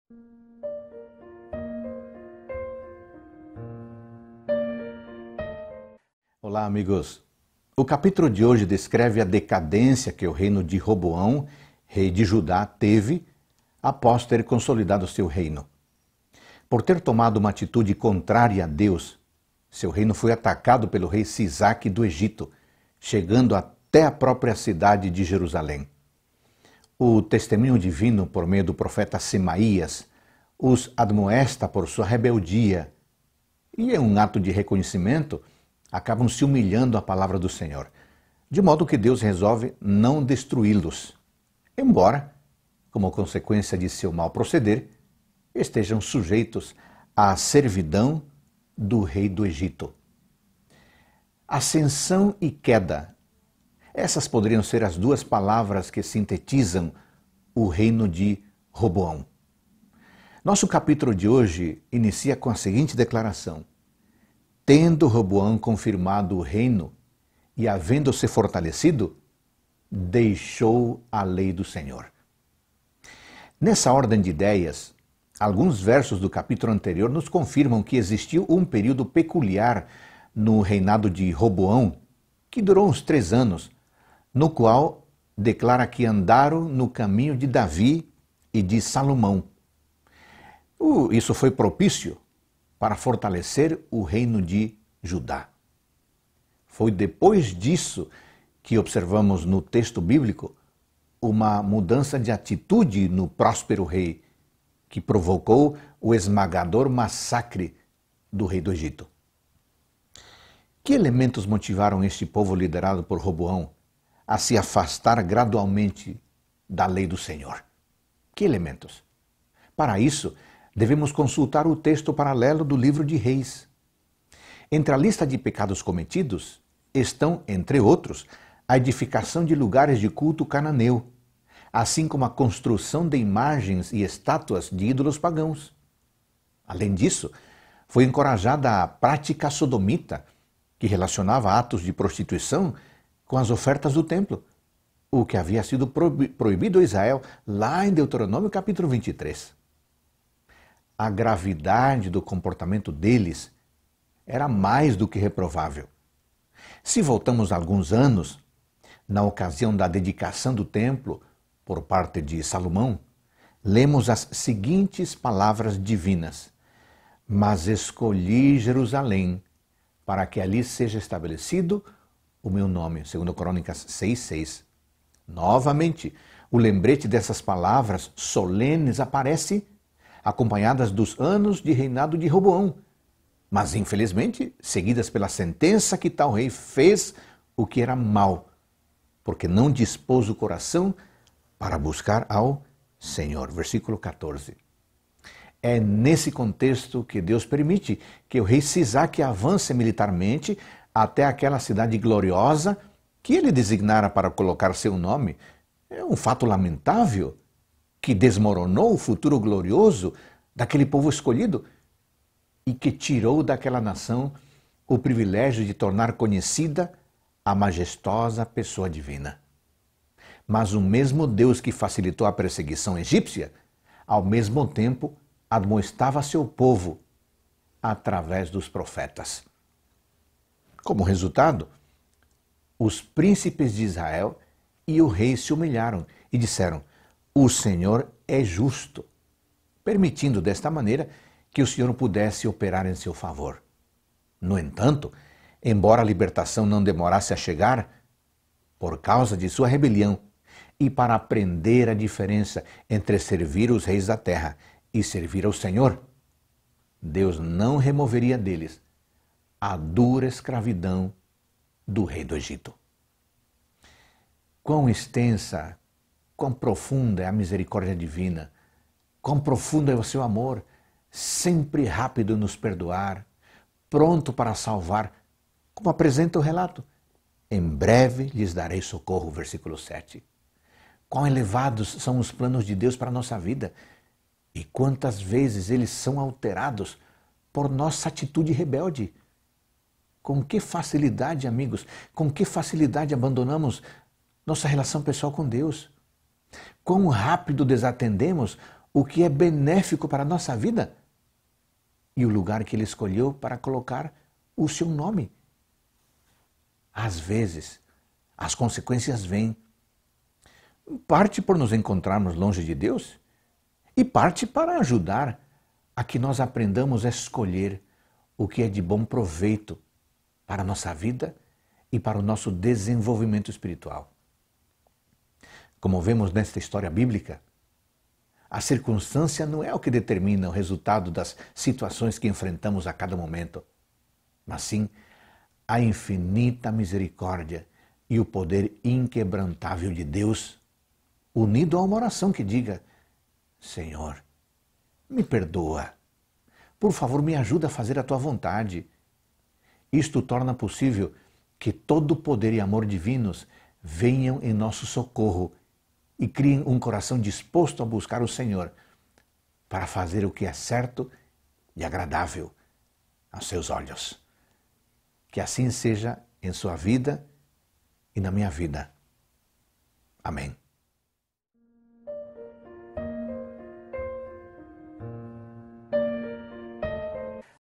2 Crônicas 12 - Reavivados Por Sua Palavra
Quão altos são os planos de Deus para nossa vida, e quantas vezes eles são alterados por nossa atitude rebelde.